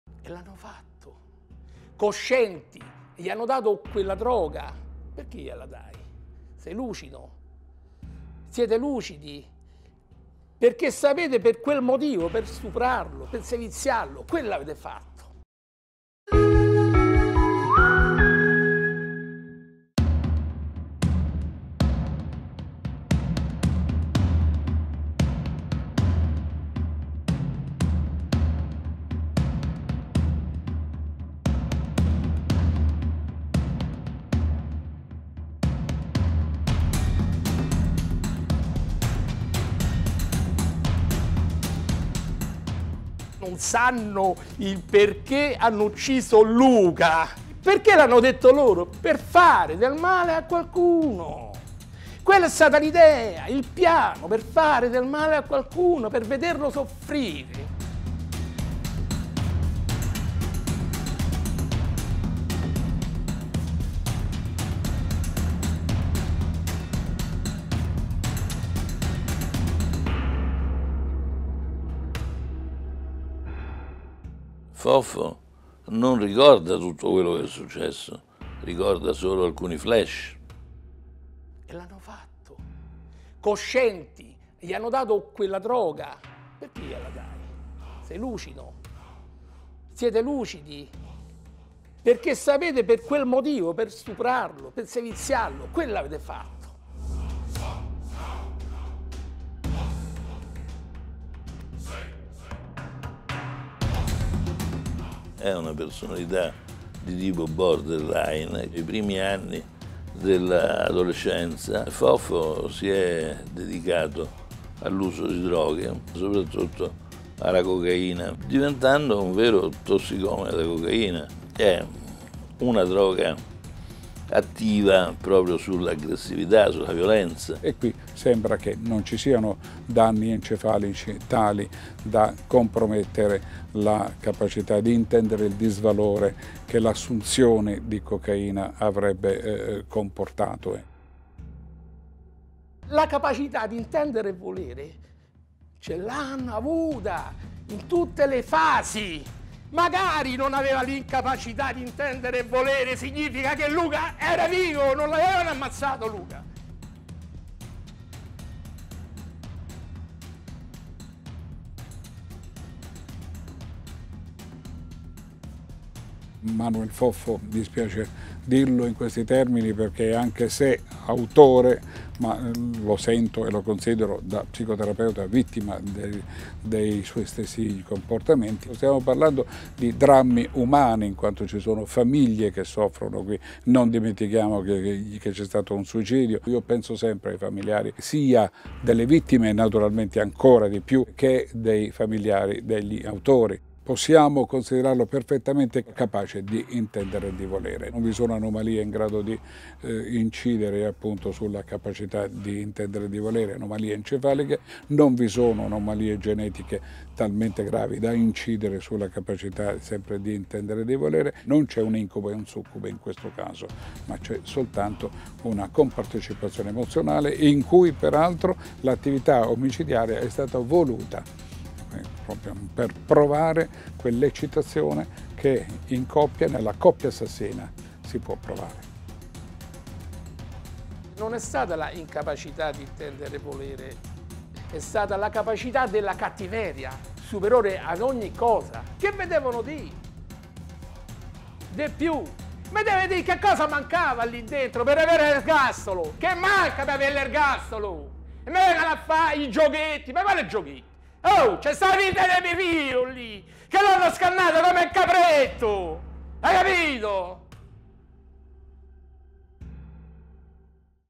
Omicidio Varani, il papà: "Chi ha stuprato e massacrato mio figlio a martellate era sano di mente"
Manuel Foffo era in grado di intendere e di volere quando massacrò Luca Varani nel suo appartamento al Collatino, insieme a Marco Prato (morto suicida in cella alcuni mesi dopo l'omicidio).
L'imputato era quindi 'sano di mente'  secondo il risultato della perizia psichiatrica disposta dalla Corte D’Appello.
'Non c'era l'incapacità di intendere e di volere ma la capacità di una cattiveria sopra ogni cosa' tuona il padre di Luca, Giuseppe Varani. L'uomo continua a lottare per avere giustizia per il figlio brutalmente ucciso e non nasconde che avrebbe voluto l'ergastolo. Manuel Foffo invece ha chiesto il rito abbreviato e perciò è stato condannato a 30 anni di reclusione in primo grado. Pochi per il padre di Luca, che chiede però che non ci siano ulteriori sconti.
Il consulente di Manuel Foffo, il professor Francesco Bruno, ha spiegato ai nostri microfoni perché invece il suo assistito non sarebbe stato in grado di intendere e di volere. 'Foffo non ricorda quello che è successo, ricorda solo alcuni flash - precisa il perito - ci troviamo di fronte ad un ragazzo con una personalità di tipo borderline e che si è dedicato al consumo di droga fin dai primi anni dell'adolescenza'.
Foffo, secondo il professor Bruno, sarebbe diventato un vero tossicomane da cocaina, una sostanza attiva proprio sull'aggressività.
Non è dello stesso parere invece il consulente della famiglia Varani, il professor Vincenzo Mastronardi: 'Non ci sono anomalie encefaliche in grado di incidere sulla capacità di intendere e di volere